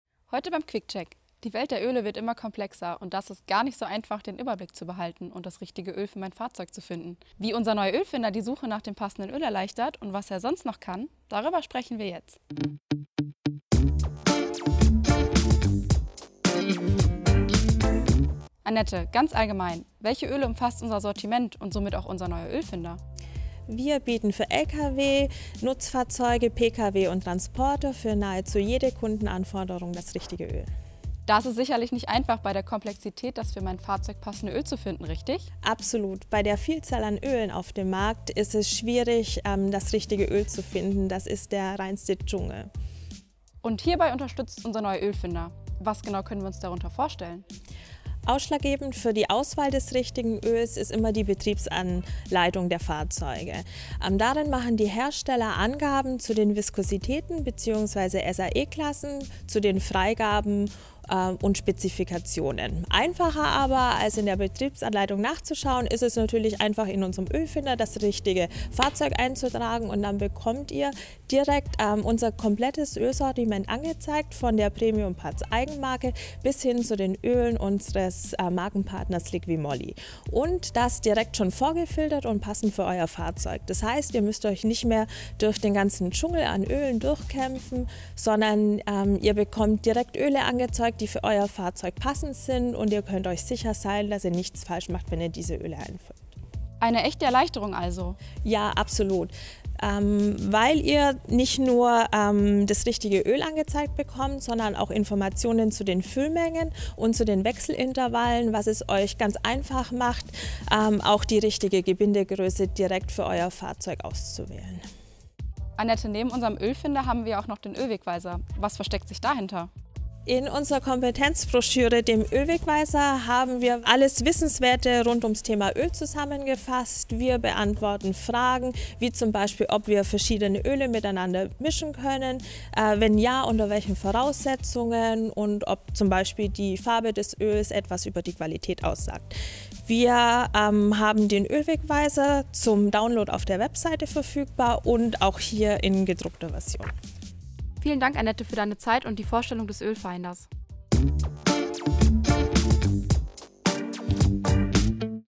Oil Finder I Quick-Check from EUROPART
In our new Quick-Check, you can find out how our oil finder can help you find the right oil for your vehicle. Whether oils can be mixed with each other and whether the colour can tell you something about the quality of the oil, you can find out in our oil guide

-- Chapter --
00:00 Intro
00:23 Our oil range
00:35 Why you should use our oil finder
00:47 How to find the right oil for your vehicle
01:40 Filling quantity and change intervals
02:00 Our oil guide
02:27 Outro